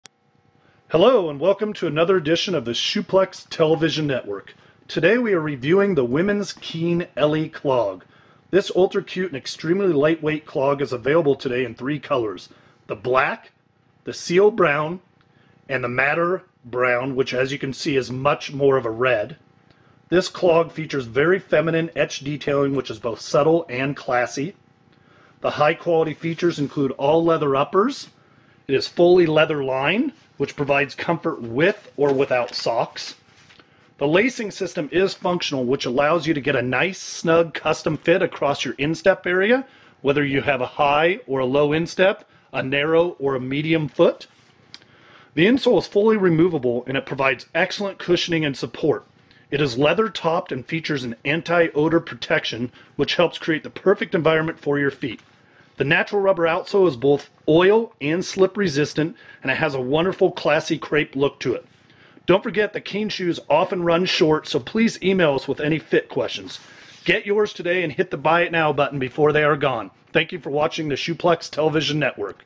Keen-Ellie-Clog-black-seal-brown-madder-red.MP4
Welcome to theShoePlex Television Network.  Today we review the Keen Women's Ellie Clog.  We have available in black, seal brown, and madder brown (red).  A fantastic low-backed comfort clog featuring a removable insole, full leather linings, leather uppers, a natural rubber outsole that is both non-marking and oil resistant.  Get your Keen Eliie clogs today before they sell out!